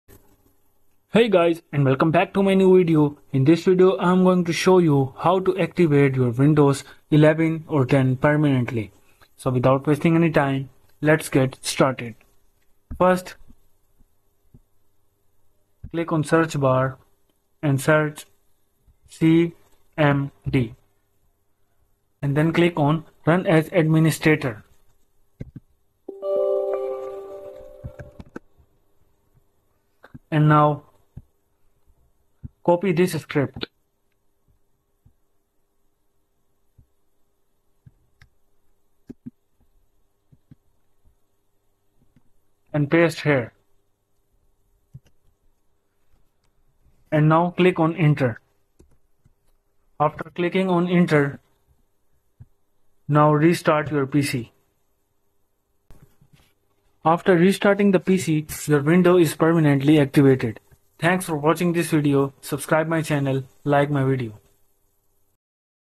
Activate Windows 10 And 11 Permanently (2025)
Activate windows 10 and 11 permanently || Windows 10 free activation in 2024

In this video I'll show you how to activate Windows 10 and 11 permanently. The method is very simple and clearly described in the video. Follow all of the steps & activate windows goto settings to activate windows 10 remove,how to activate windows 11 activate the